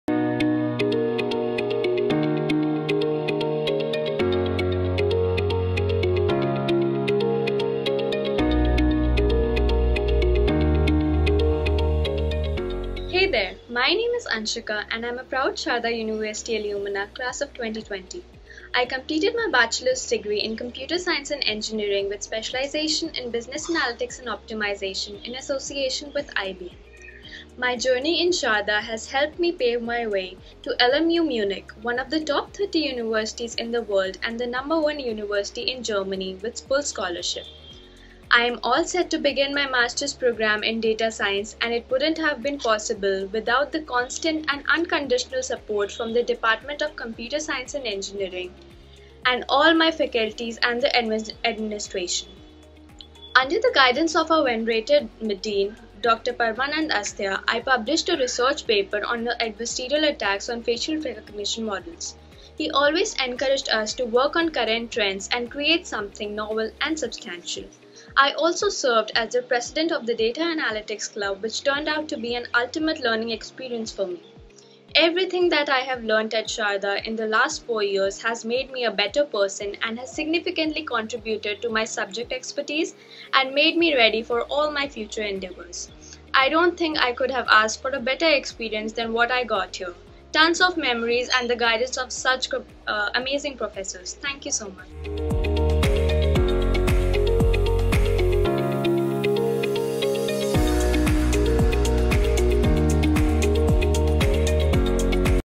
Sharda University | Anshika - B.Tech (Computer Science Engineering) | Success Story
Ms.Anshika, a B.Tech (Computer Science Engineering) student from ShardaUniversity describes her college experience to be a great contributor in helping her find her mark. The friendly attitude of the professors and their willingness to always offer a helping hand has helped her find her way to the world's #32 ranked university- LMU Munich, at 100% scholarship for her Mastersprogramme. She also adds that the diverse range of international students on campus meant a real cultural experience for her.